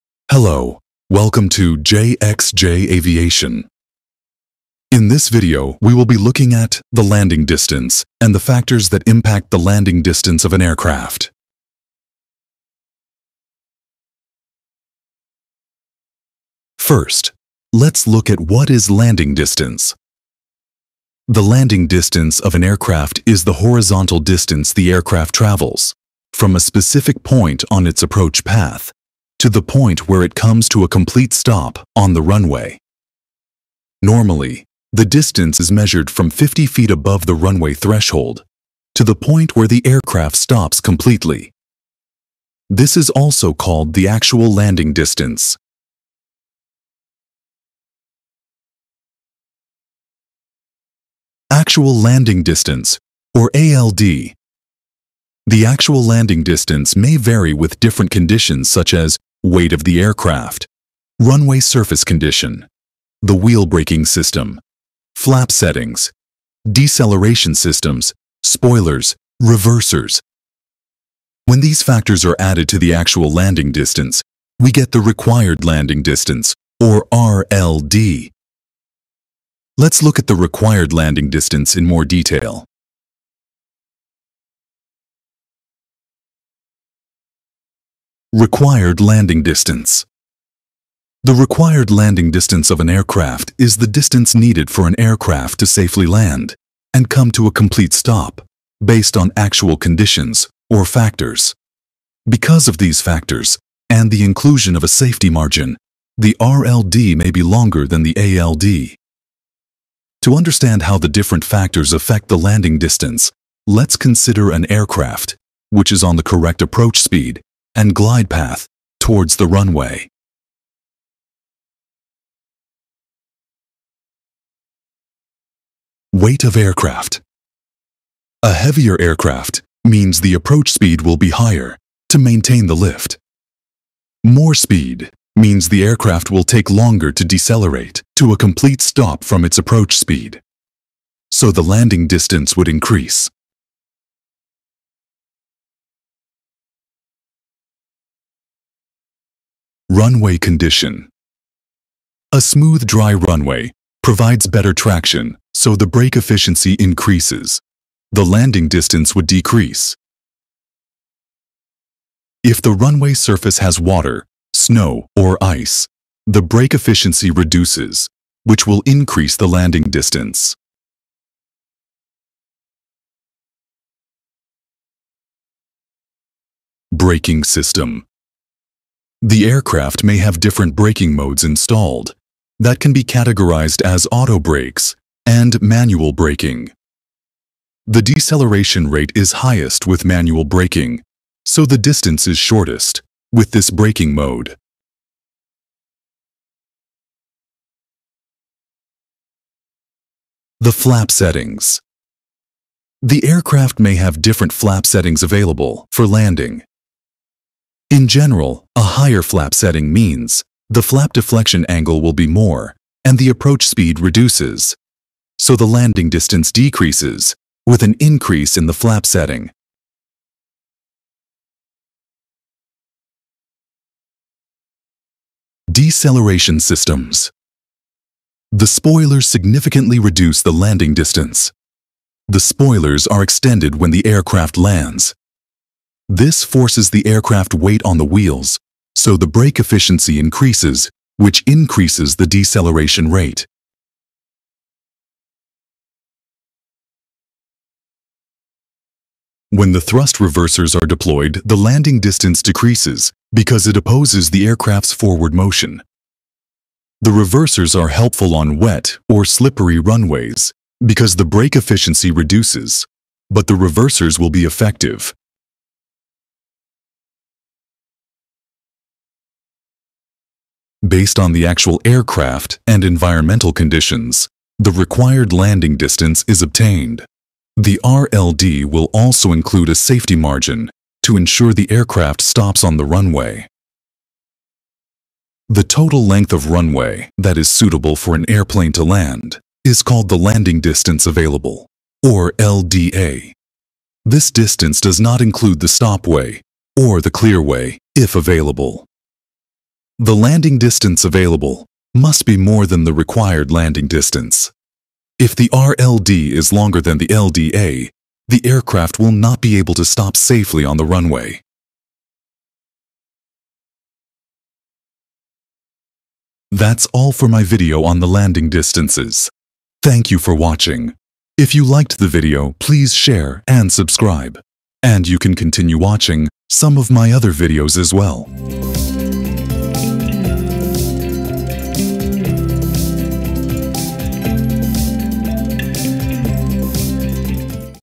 Understanding Landing Distances and the factors that impact the Landing Distance | ALD | RLD | LDA
Hi. In this video we look at what is the actual landing distance, which is taken in ideal conditions. We then look at the required landing distance, which is based on actual conditions of the aircraft and the environment, along with a safety margin. We also look at what is the available landing distance, and how should it compare with the RLD.